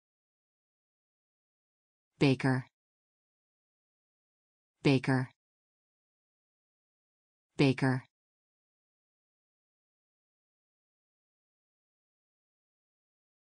How to say baker in English
A short video showing how to pronounce baker.